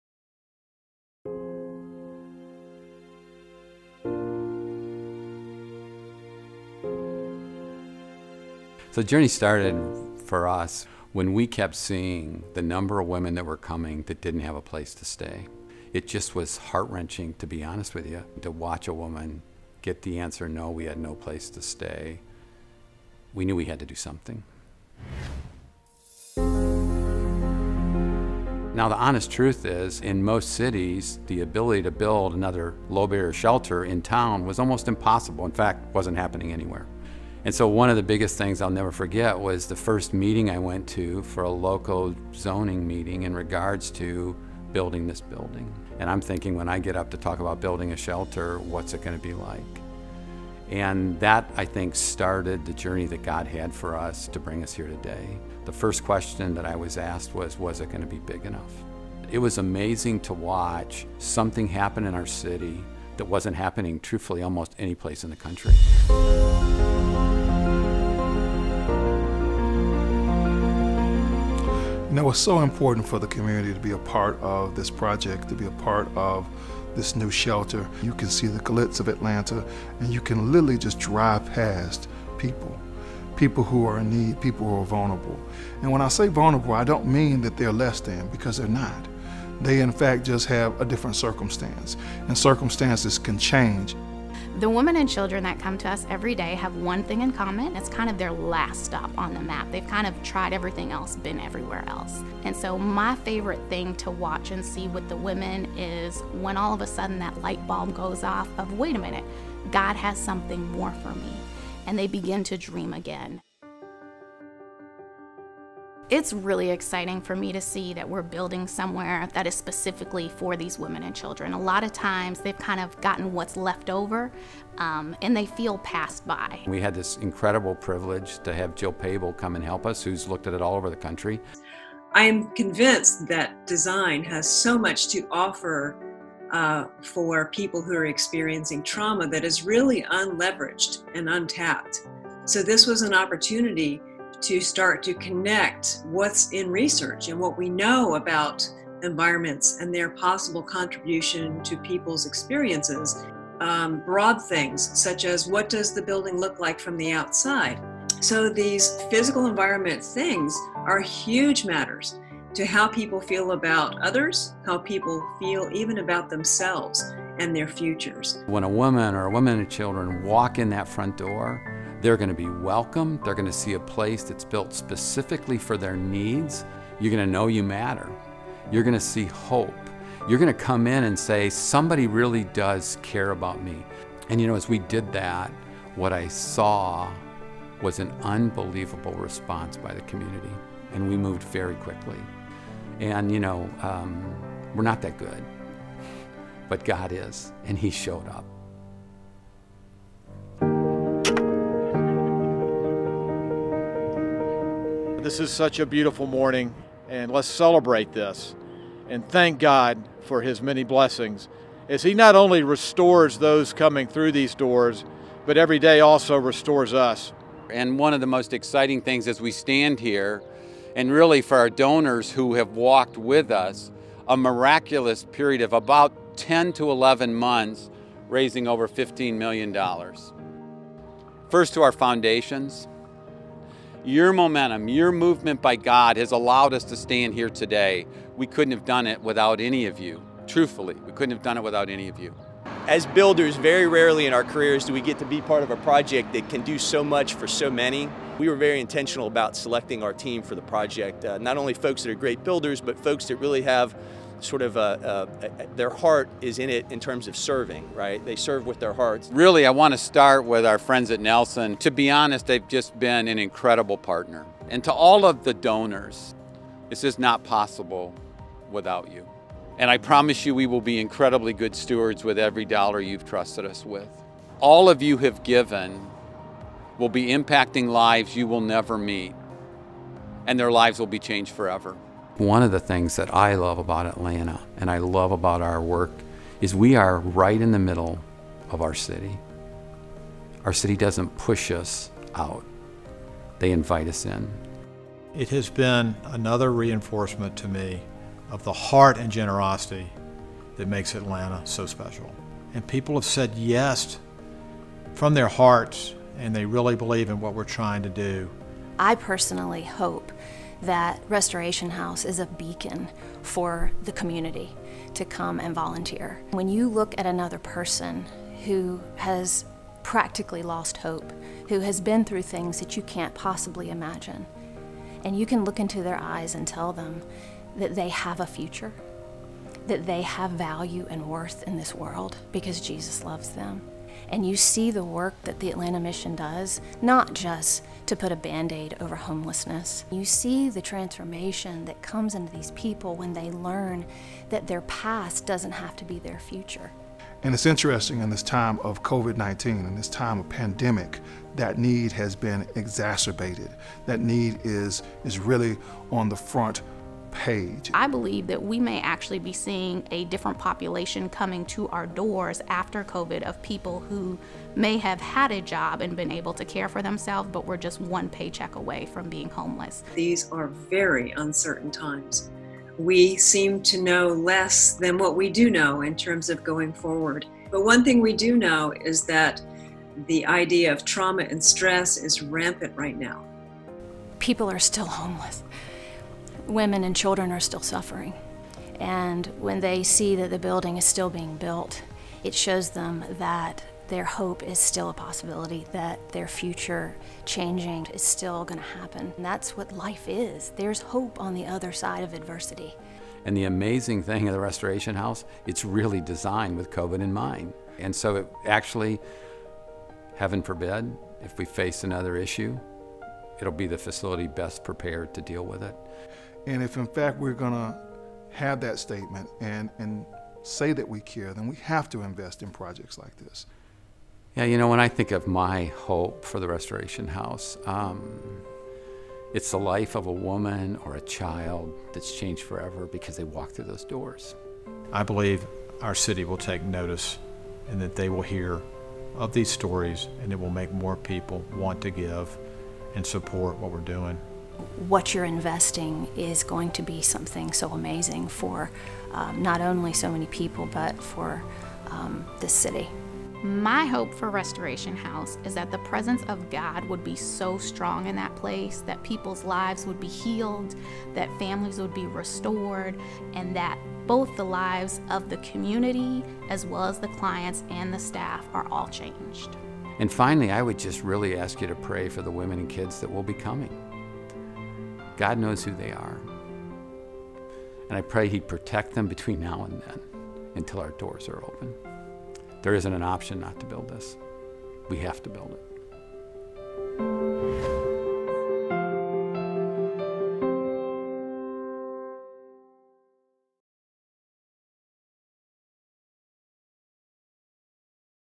Atlanta Mission-- Restoration House
Construction is underway for Restoration House! We celebrated the groundbreaking of our new Ethel Street facility with our community partners in June 2020. Learn more about Atlanta’s new, innovative, low-barrier shelter for women and children opening summer 2021.